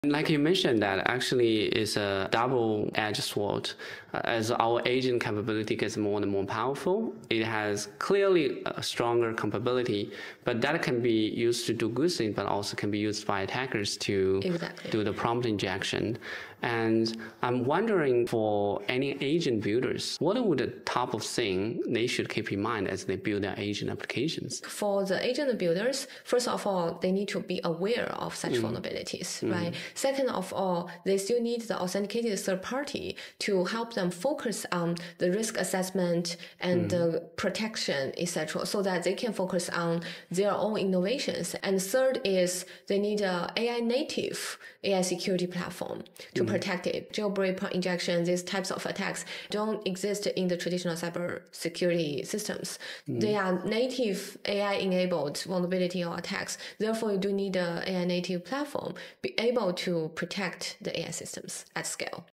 The Smarter Your AI Agent, The Easier It Is to Hack
Here's the problem nobody talks about: The better your AI agent gets at following instructions, the better it gets at following malicious instructions.
Bo Li calls it a "double-edged sword"—and it's creating a critical security challenge for every company building agents.
The paradox:
→ You want: Agents that follow complex instructions
→ You get: Agents vulnerable to sophisticated attacks
→ The result: More capability = more attack surface
Three must-dos for agent builders:
1. Awareness is baseline
Understand that agent vulnerabilities are fundamentally different from traditional security risks.
2. Focus on innovation, not security firefighting
Use third-party risk assessment so your team builds, not defends.
3. Use AI-native security platforms
Prompt injections and jailbreaks didn't exist in traditional cybersecurity. You can't protect AI systems with non-AI tools.
The hard truth: Traditional firewalls don't stop prompt injections. Traditional antivirus doesn't catch jailbreaks. AI-native threats need AI-native defenses.
Building AI agents? Your security strategy needs to evolve as fast as your capabilities do.